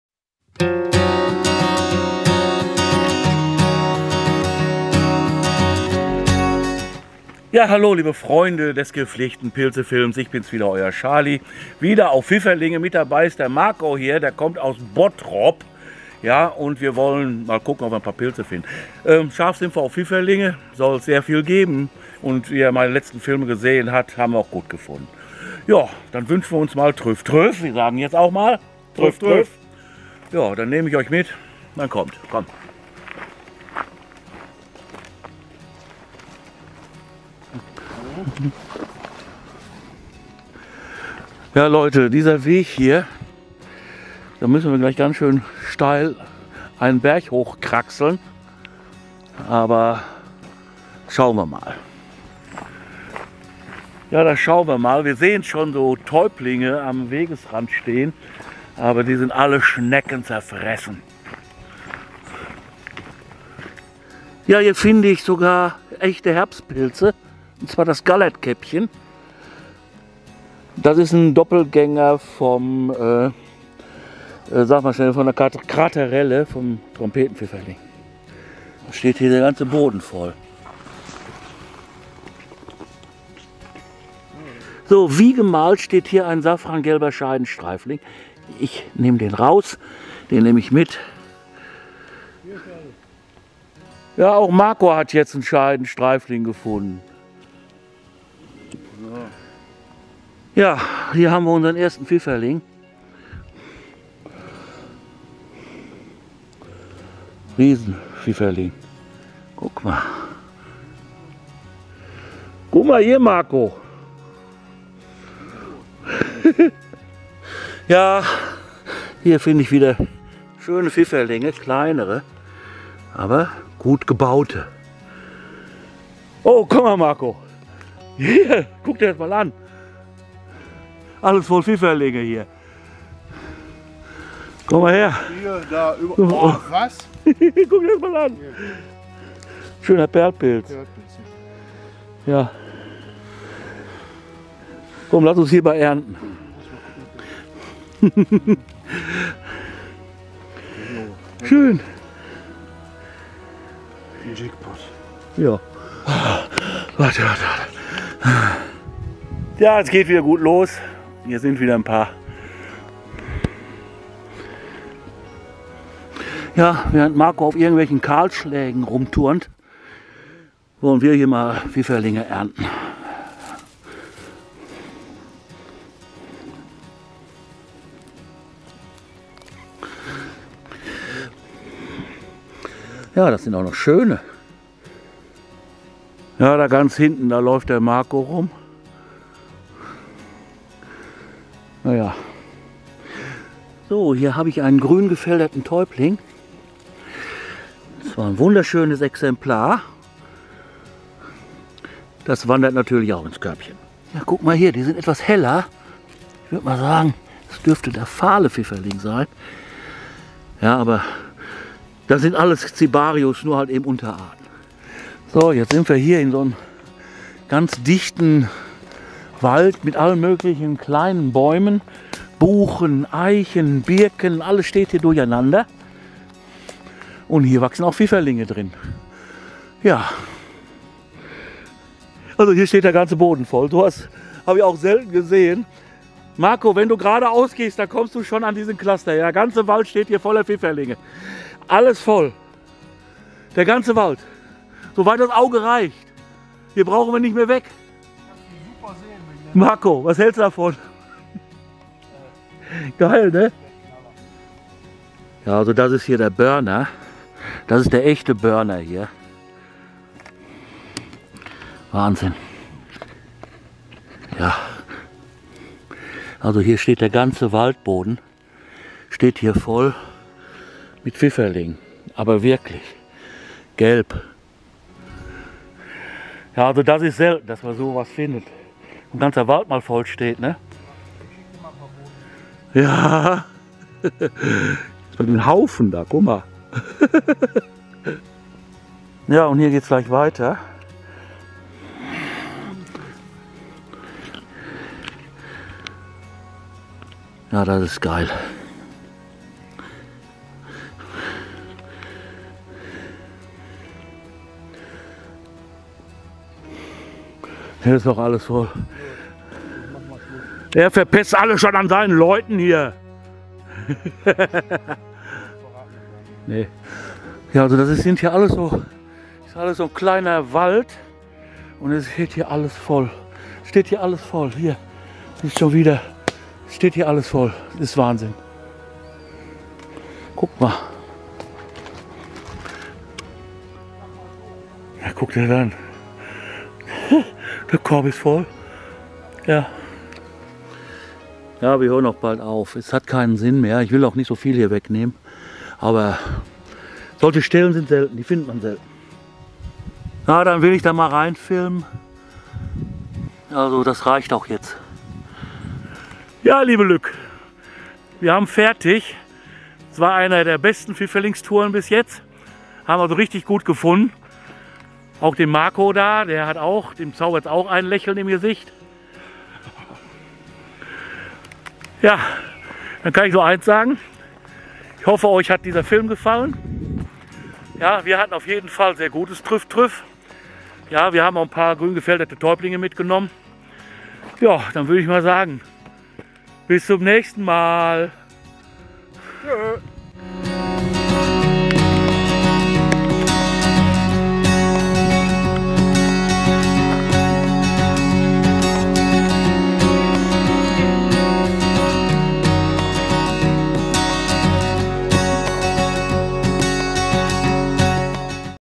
Pfifferlinge - Wie Erdbeeren pflücken ! (2024)
Was für ein Tag! Marco aus Bottrop war zu Besuch, um ein paar Pfifferlinge zu suchen und ich war schon am überlegen, wo man denn eigentlich noch hingehen könnte. Ich war in den letzten Tagen ja bald täglich unterwegs und überall habe ich gute Funde gemacht. Mein Heimatort "Kierspe" ist gottlob von Wäldern umgeben und da kann man in der Regel auch überall hinfahren. Es war allerdings Sonntag und an den vorherigen Tagen waren bestimmt auch andere Leute wegen der Pilze in den Wäldern. Deshalb dachte ich mir ,dahin zu fahren, wo ohne Ortskenntnis selten jemand hinkommt.
Um an diese Stelle zu kommen, galt es wiederum steile Hänge zu erklimmen und durch teilweise unwegsamen Wald zu brechen. Dem Marco hat es schwer gewundert, das ich diese steilen Hänge mit meinen fast 67 Jahren so mühelos wie eine Gams erklimmen konnte. Dort, wo die Borkenkäfer-Kahlschläge waren, war es teilweise ein echter Kampf mit dem wuchernden Ginster- und Brombeergestrüpp. und Macheten hatten wir nun einmal nicht dabei. In den erreichten Buchen/Eichen-Mischwäldern wurden wir aber in Punkto "Pfifferlinge" nicht enttäuscht. Überall gab es Cluster von Pfifferlingen. In einem Bestand mit kleinen morschen Borkenkäferfichten mit eingestreuten Buchen, Eichen und Birken erreichten wir letztendlich einen Fundpunkt, wo der ganze Waldboden mit Moos und mit Pfifferlingen bedeckt war. Es waren so viele, das es quasi so wie "Erdbeeren pflücken" auf einem Feld war, so dicht standen die Fruchtkörper der begehrten Pilze. So haben wir beide unsere Körbe voll gemacht und konnten von der Natur reich beschenkt den Heimweg antreten. Im Moment gibt es eine echte Schwemme von Pfifferlingen und das nun das vierte Jahr in Folge. Vor dem Borkenkäfer waren derartige Funde in unseren Wäldern eher selten und Pfifferlinge gehörten mehr zu den Zufallsfunden. Möge es noch lange so gehn. In diesem Sinne...TRÜFF,TRÜFF und gute Funde.